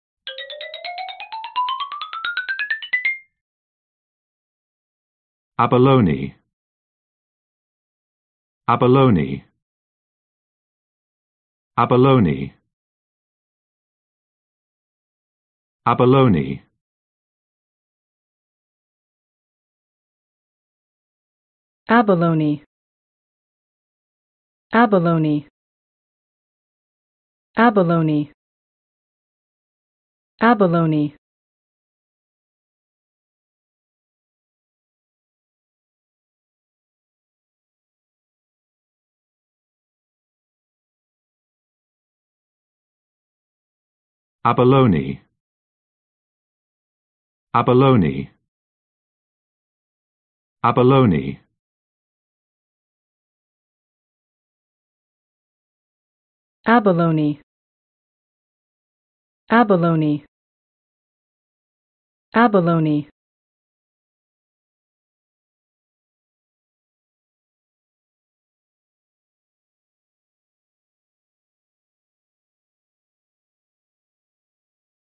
英単語 abalone 発音と読み方
英単語abaloneの発音動画です。

イギリス英語での発音記号は「æb.ə.ˈləʊ.ni」でアメリカ英語での発音記号は「-ˈloʊ.ni」です。